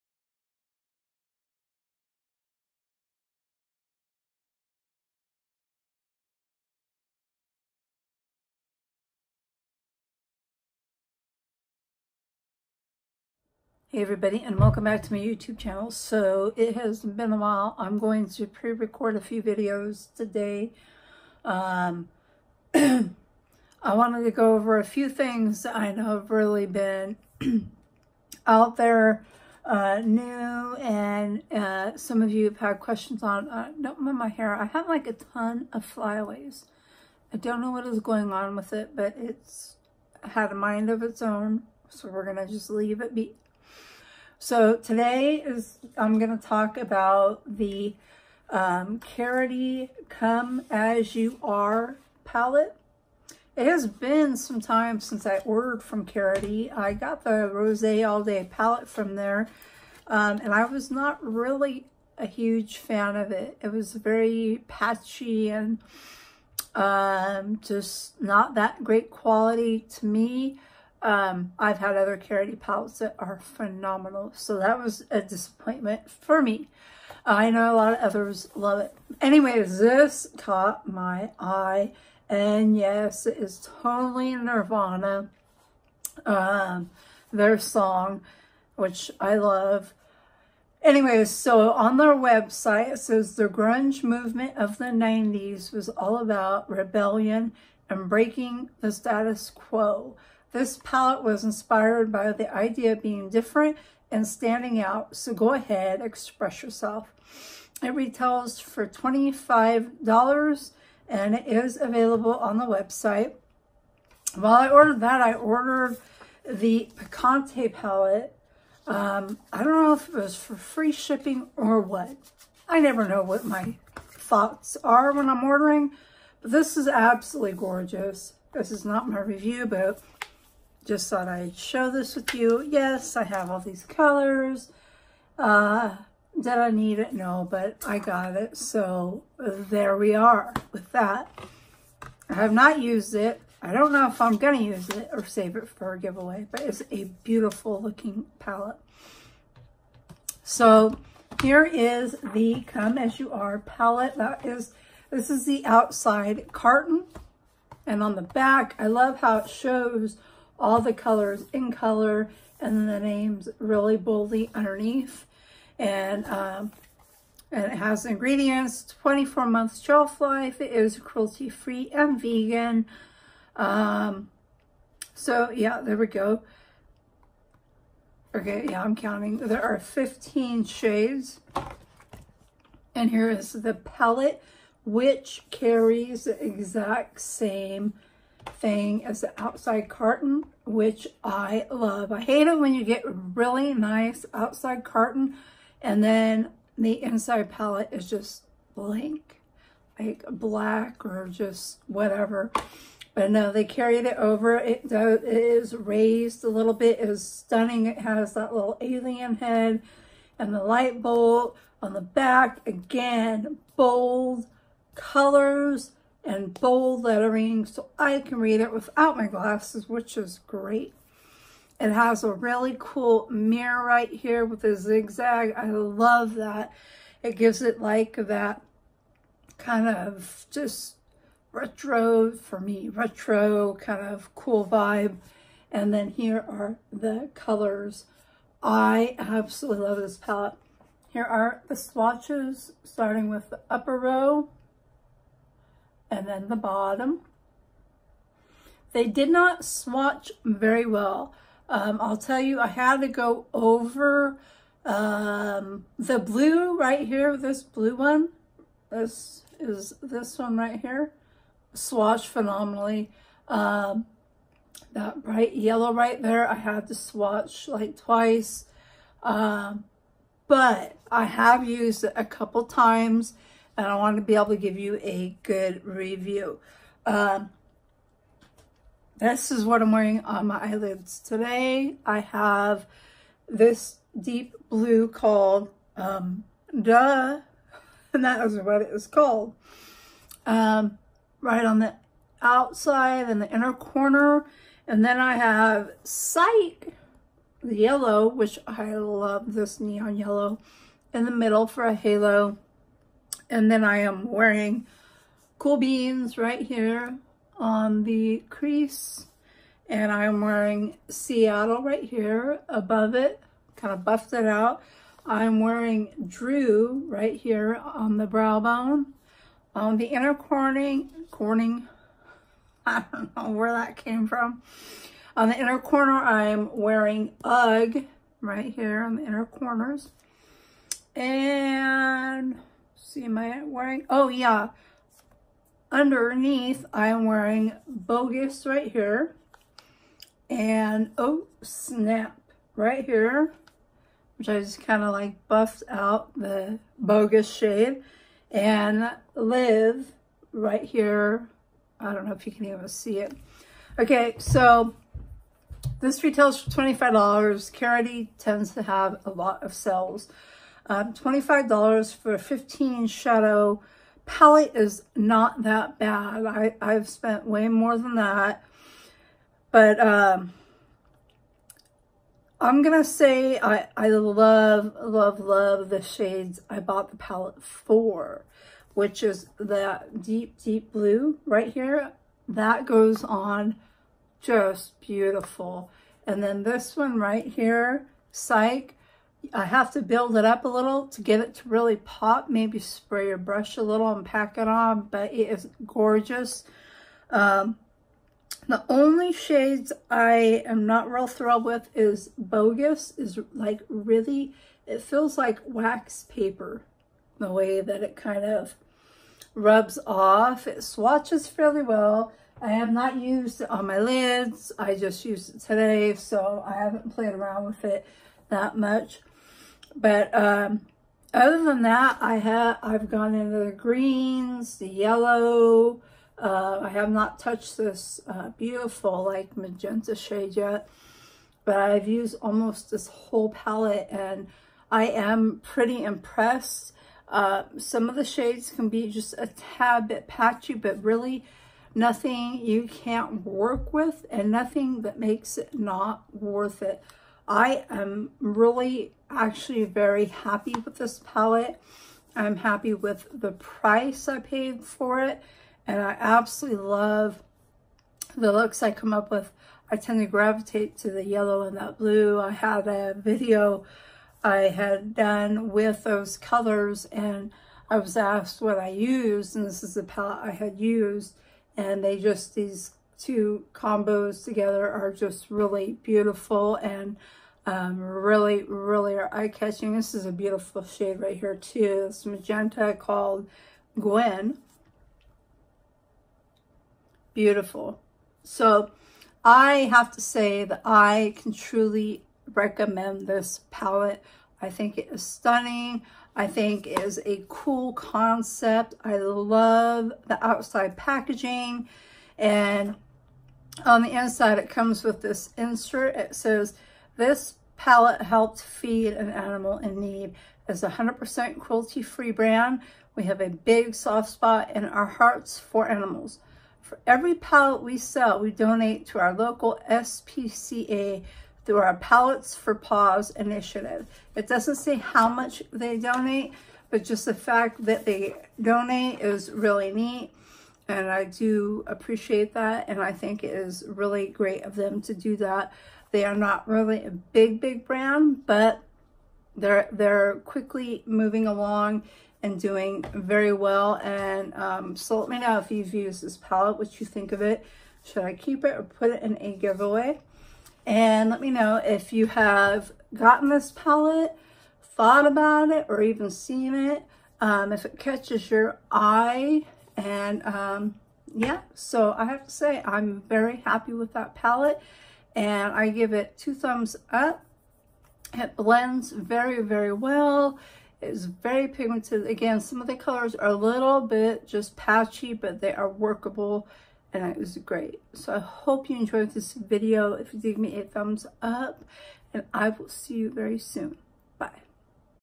Karity Come As You Are eyeshadow palette!!
Swatches and full reviews on this Karity palette!! Worth it or Not? I forgot to mention there's not a lot of fallout with most of the shadows!